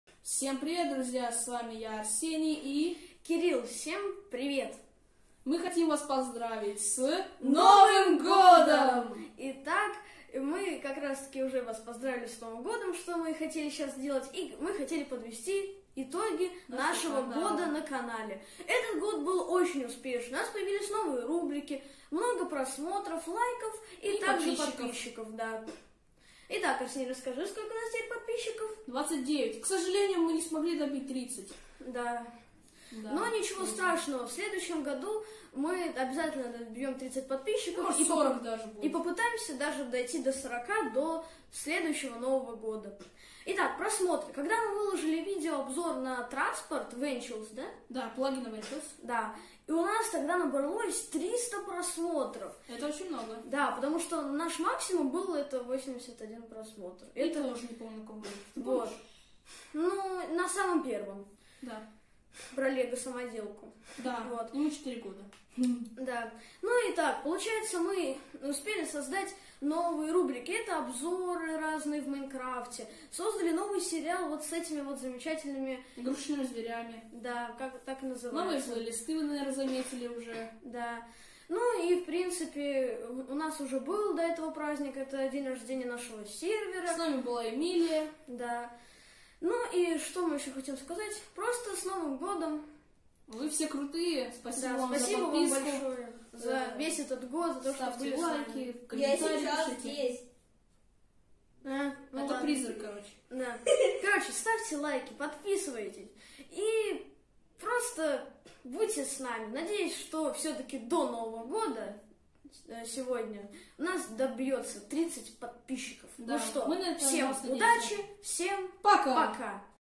С новым годом вас, подписчики!
В этом видео я с Кириллом, подведём итоги нашего канала за этот замечательный год!
Также мы забыли добавить что график наших роликов измениться, мы будем выпускать ролики раз в 2-3 недели.